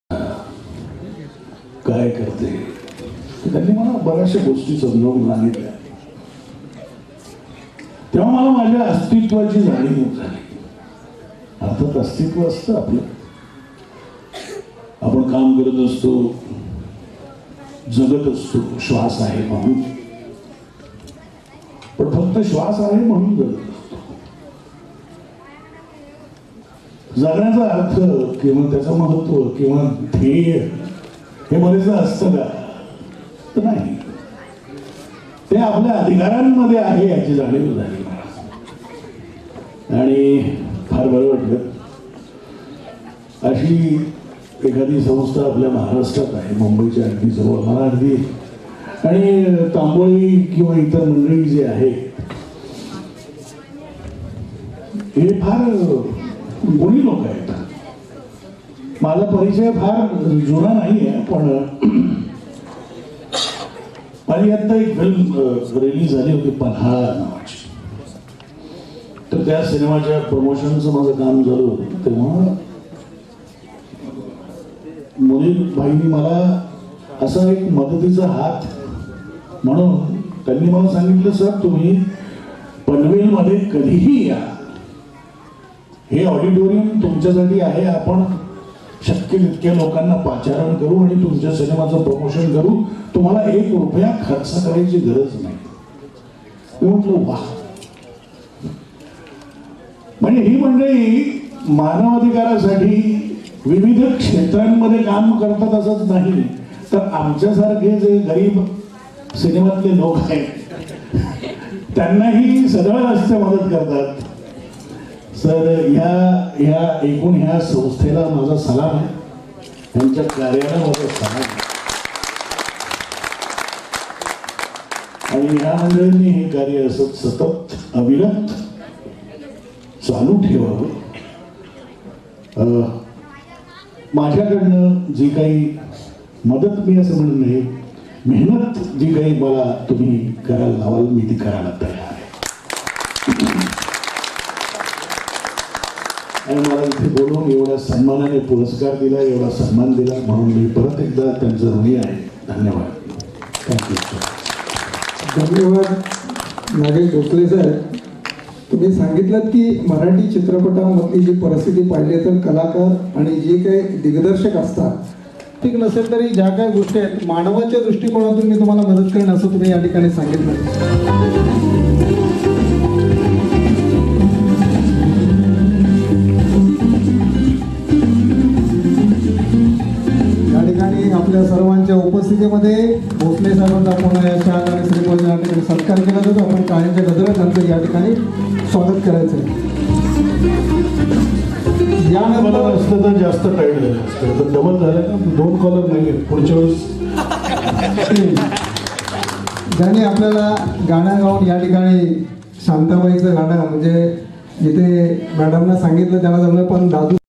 Actor Director Nagesh Bhosle Sir Speech 2nd IHRA Festival
2nd I.H.R.A FESTIVAL World Human Rights Day Celebration Program (A Grand Charity Entertainment Event to Support Project Shiksha)